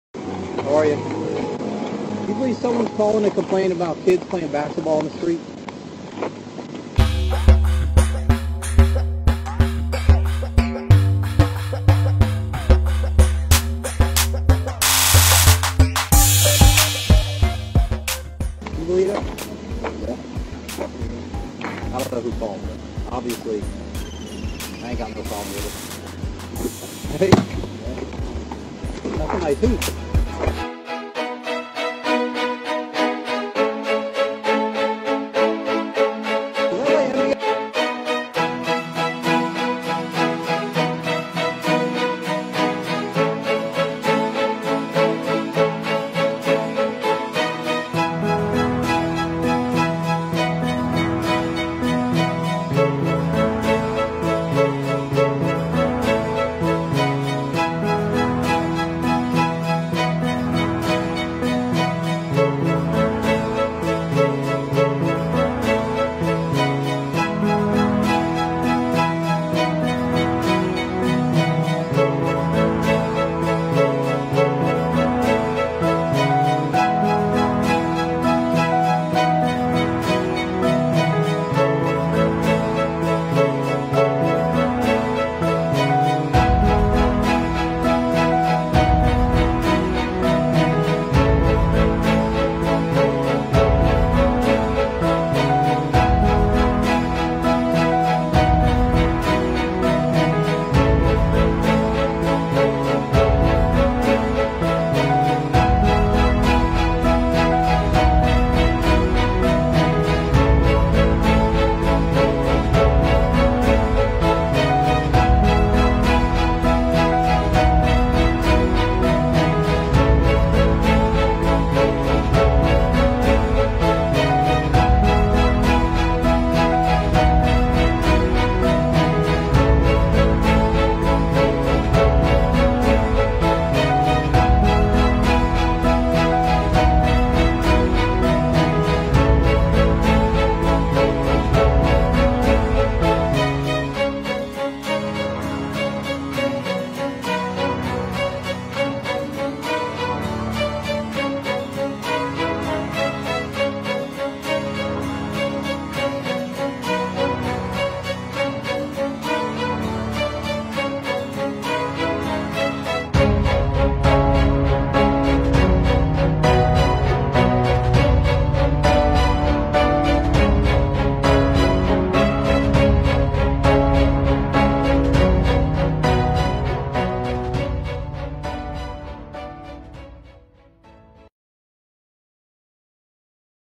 The amazing reaction of an officer after arriving at the “crime scene” | Positive
The police station in Gainseville, Florida, received a call about some loud kids playing basketball in the street. So an officer was immediately sent to see what was going on. But what he did next to control the situation, left the whole community speechless! “This is the right way to do rounds!” claimed some local neighbors...